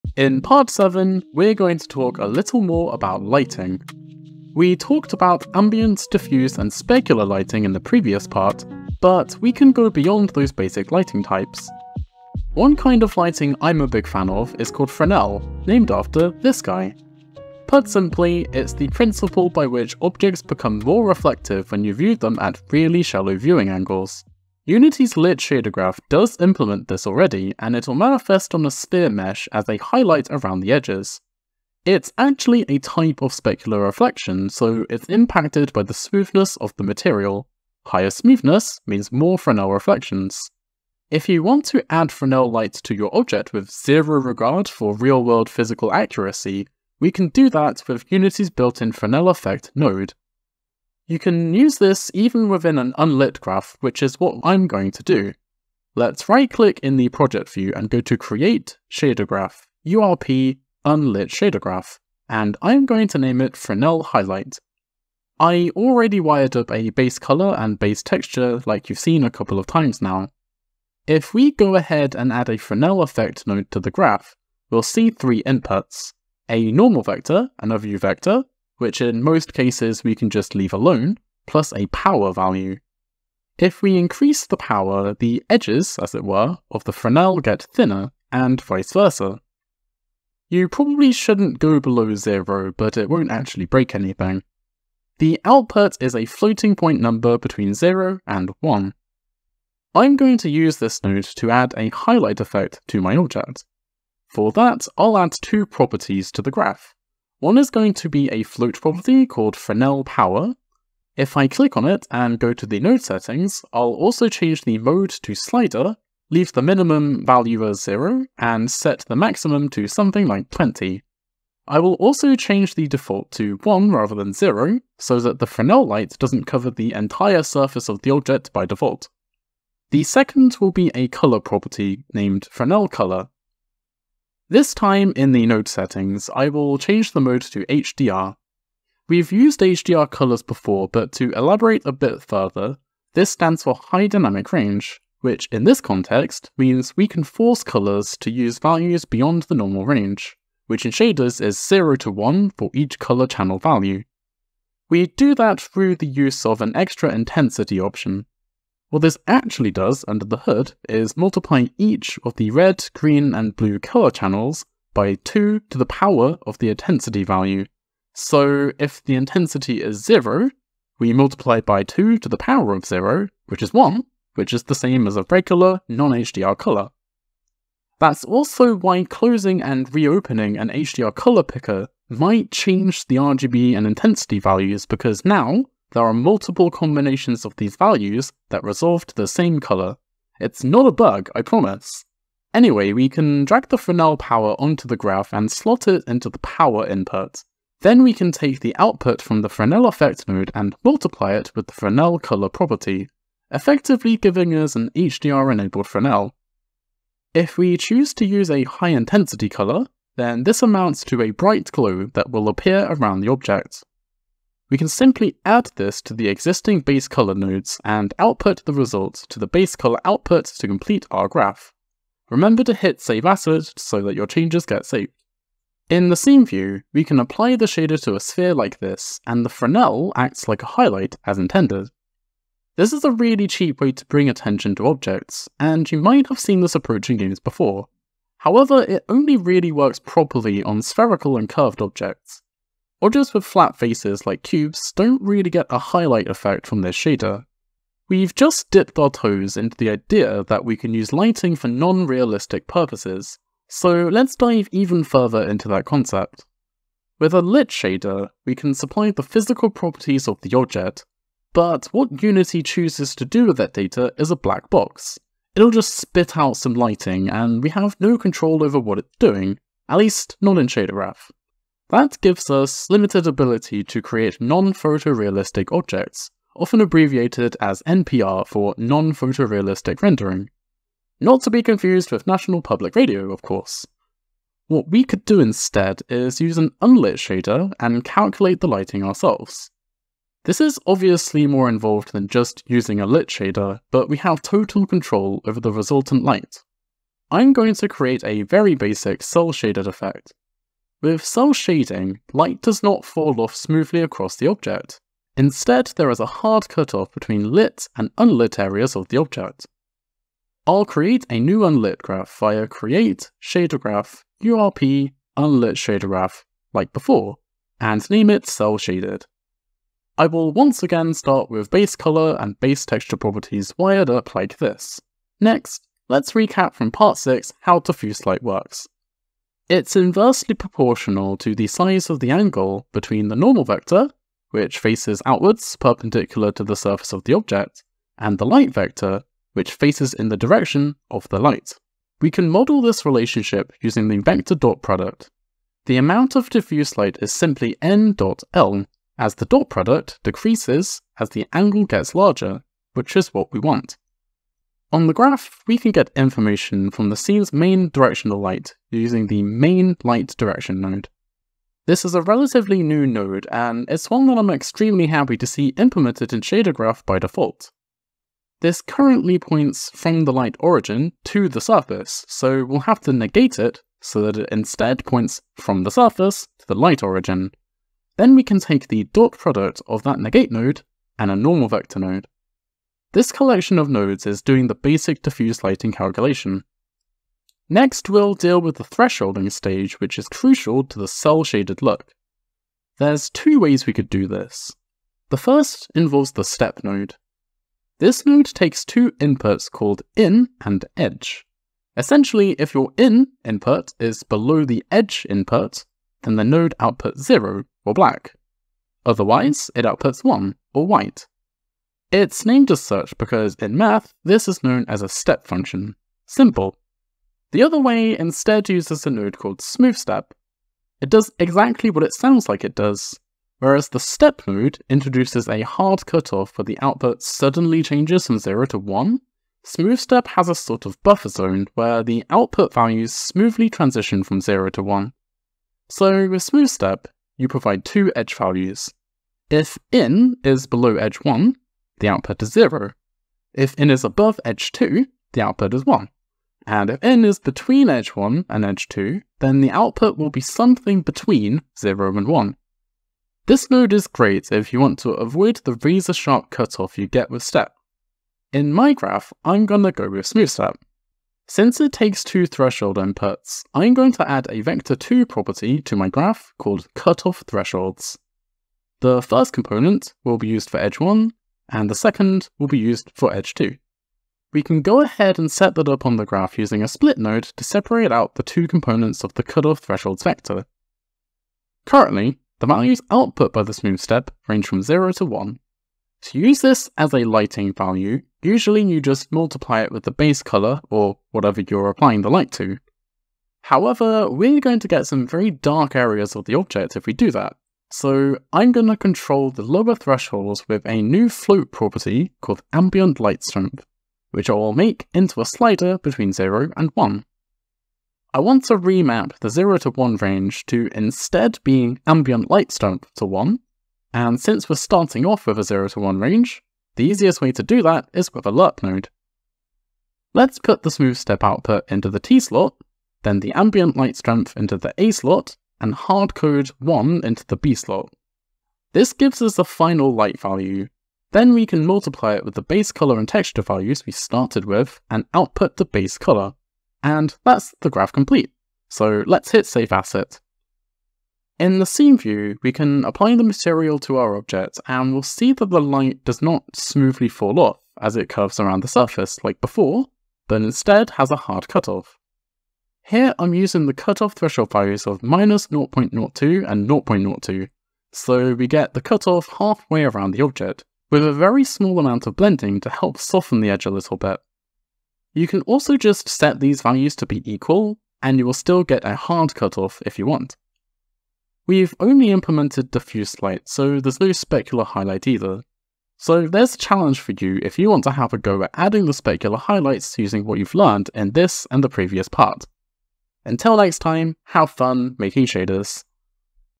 Unity Shader Graph Basics (Part 7 - Custom Lighting)
Although the Unlit shader type doesn't automatically add lighting to objects, we can implement our own lighting however we want. In this tutorial, learn how to add a Fresnel highlight to the edges of objects and create a basic cel-shaded effect which implements and augments diffuse lighting.

I'm using Unity 2022.3.0f1, although these steps should look similar in previous and subsequent Unity versions.